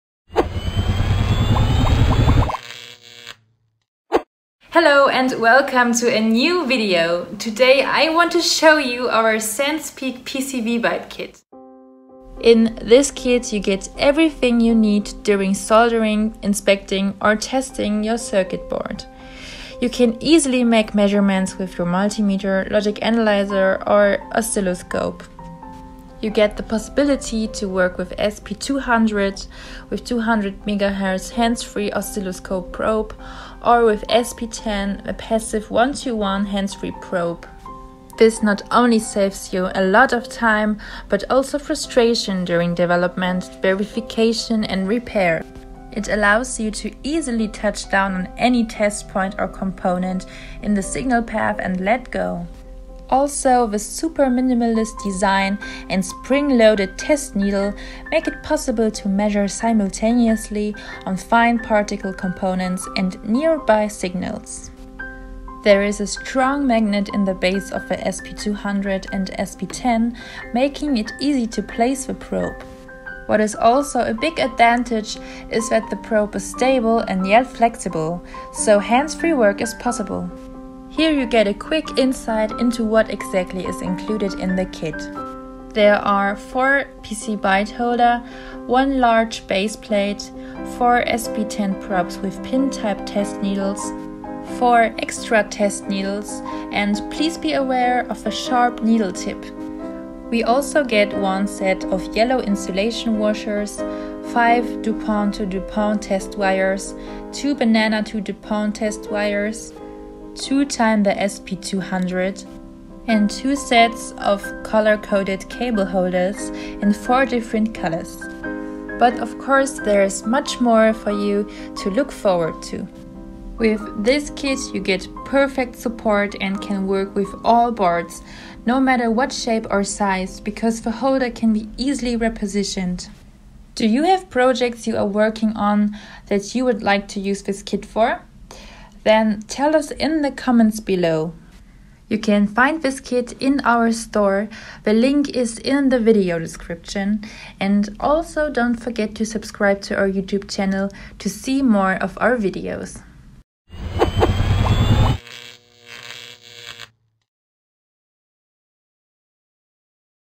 Overview: Sensepeek PCBite Kit with 2x 200 MHz and 4x SP10 handsfree Probe
SP200 – 200 MHz hands-free oscilloscope probe
The first truly hands-free 200-MHz 10:1 oscilloscope probe! Makes instant measurements or long triggering sessions a breeze.
No more soldering wires to connect your probe or complicated tools to set up. Just position the probe needle on any test point or component in the signal path and release.
Saves time and frustration during development, verification, and repairs.
The minimalist design and the spring-loaded test needle make it possible to simultaneously measure fine pitch components and nearby signals.
The ground probe connection has a detachable quality hook and the cable fits directly on a 2.54 mm (0.100ʺ) pitch pin header.
The SP200 comes with a powerful magnet in the base, as for all PCBite probes and holders, which makes the probe easy to place and reposition.
This probe is perfect for those who already have a metal base plate and holders which is a must for the function.
SP10 – Passive 1:1 hands-free probe
A truly hands-free probe! Makes instant measurements or long triggering sessions simple.
The probe is steady but yet flexible made for instant measurements or total hands-free operations together with your multimeter, logic analyzer, or preferred tool.
The dual pin header connector fits directly on a 2.54 mm (0.100”) connector.
The SP10 comes with a powerful magnet in the base, as for all PCBite probes and holders, which makes the probe easy to place and reposition.

00:00 Intro
00:13 What's Inside
00:57 Functions
02:17 Question/Outro